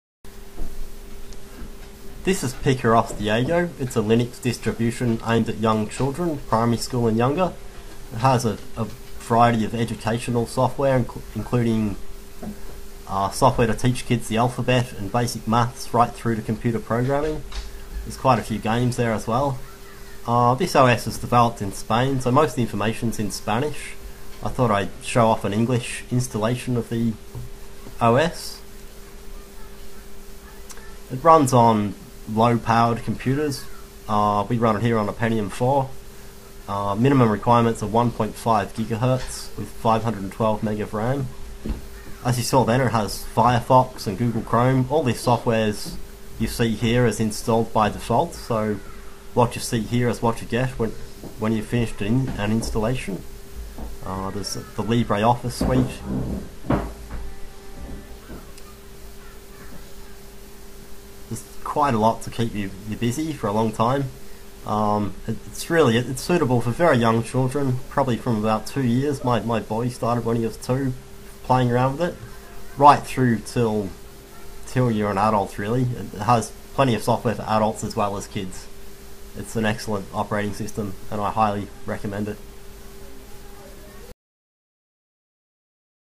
Kids Linux - PicarOS Diego English
Demo video of an English installation of the children's Linux distribution PicarOS Diego, the most awesome kids Linux. You can run the software live from the DVD if you don't want to install it or if you just want to try it out.